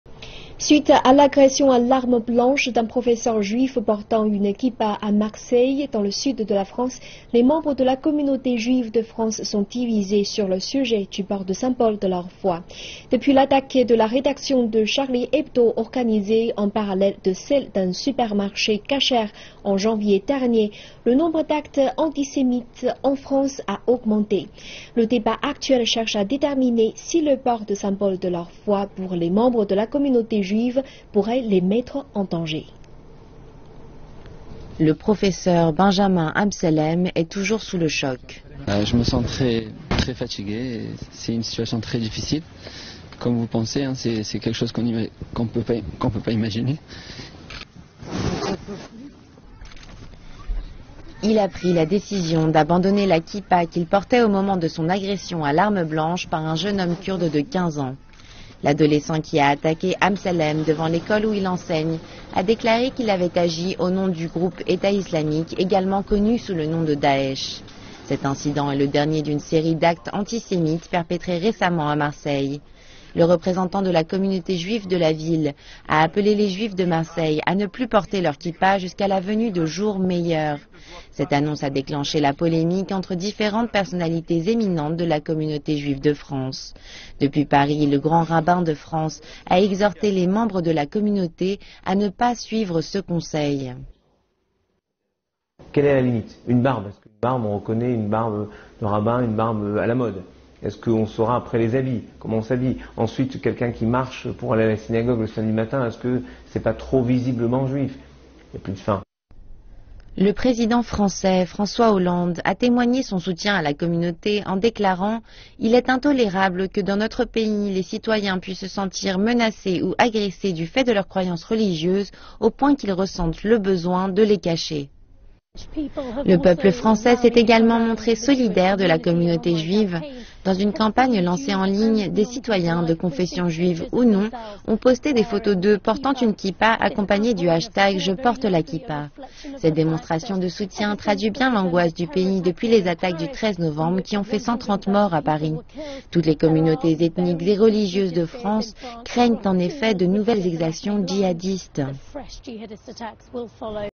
Marseille : l’attaque d’un professeur juif inquiète la communauté juive de France
Suite à l'agression à l'arme blanche d'un professeur juif portant une kippa à Marseille, dans le sud de la France, les membres de la communauté juive de France sont divisés sur le sujet du port de symboles de leur foi.
Depuis l'attaque de la rédaction de Charlie Hebdo organisée en parallèle de celle d'un supermarché kasher en janvier dernier, le nombre d'actes antisémites en France a augmenté. Le débat actuel cherche à déterminer si le port de symboles de leur foi pour les membres de la communauté juive pourrait les mettre en danger.
Le professeur Benjamin Amsellem est toujours sous le choc.